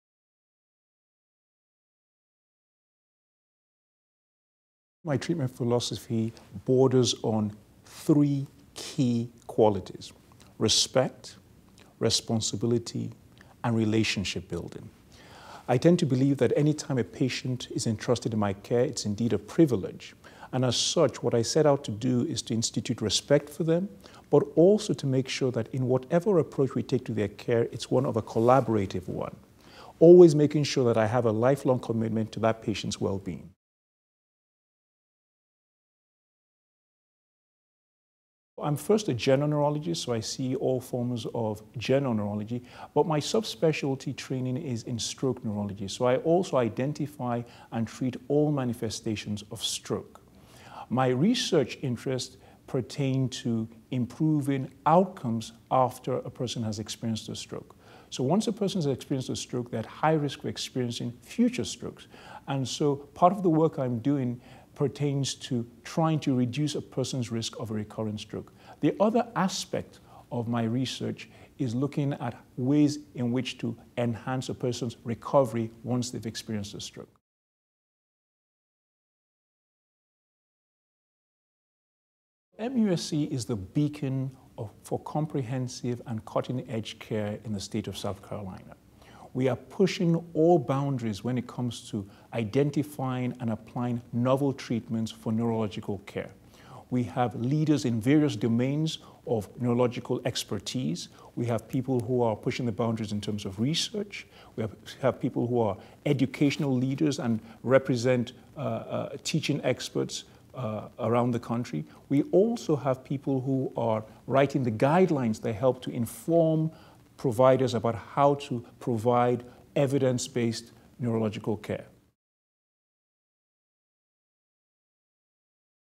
Dr. Bruce Ovbiagele, Neurology - MUSC Health
Bruce Ovbiagele, M.D., MSc, MAS, is Professor and Chair of Neurology at the Medical University of South Carolina (SC, USA). He is an elected Fellow of the American Academy of Neurology, American Neurological Association and American Heart Association Stroke Council. He has published numerous peer-reviewed papers on stroke epidemiology, stroke prevention and T2/T3 translational stroke research.

Dr. Ovbiagele also serves as a member of the American Society of Neuroimaging, and a member of the American Heart Association Council on Epidemiology and Prevention. He serves as Chair of the Minority Affairs Committee of the American Heart Association Stroke Council and is a member of the American Academy of Neurology Education Committee and Cerebrovascular/Critical Care/Interventional topics Writing Committee. Dr. Ovbiagele serves on the editorial board of the Journal of Stroke and Cerebrovascular Diseases and as an assistant editor of Stroke, and has served as a reviewer for numerous other journals. He is an active researcher, and his research portfolio has included funding from the NIH, American Heart Association, and industry. Over the course of his career, Dr. Ovbiagele has served as a mentor to numerous residents and fellows. He has served as an oral board examiner and as a member of the Vascular Neurology Examination Writing Committee for the American Board of Psychiatry and Neurology.